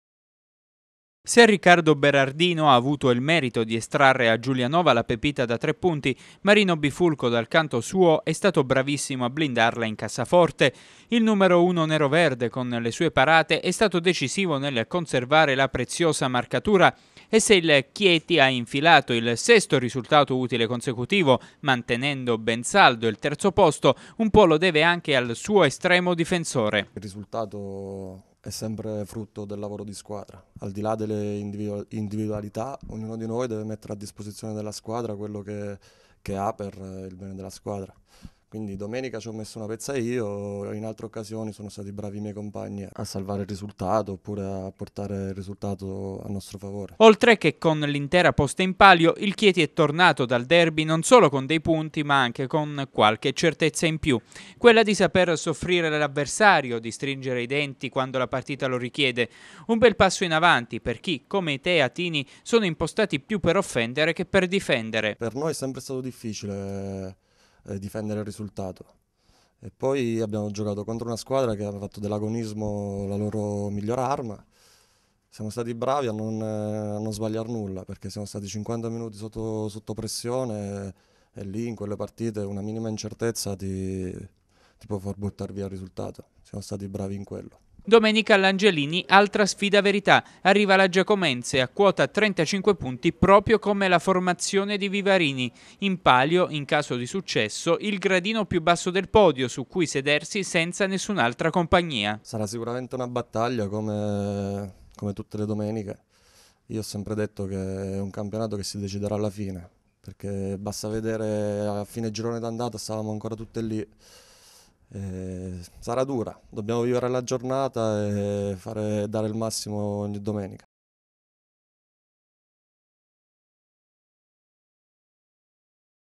Bifulco: "Le individualità al servizio della squadra"
Protagonista dell'affermazione del Chieti a Giulianova, il portiere Marino Bifulco esalta le qualità di tutto il gruppo neroverde e guarda con fiducia al prossimo scontro playoff con la Giacomense